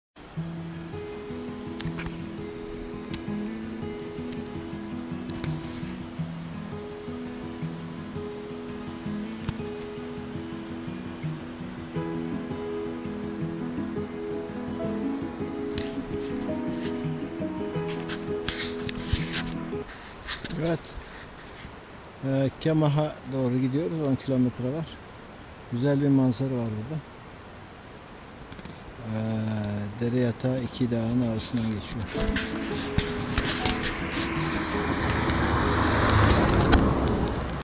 Erzincan’dan Kemah ilçesine yolculuk-2019 Büyük Anadolu turu-Journey from Erzincan to Kemah (739)
Erzincan’dan Kemah ilçesine yolculuk-2019 Büyük Anadolu turu-Journey from Erzincan to Kemah village-2019 Great Anatolia Tour (739)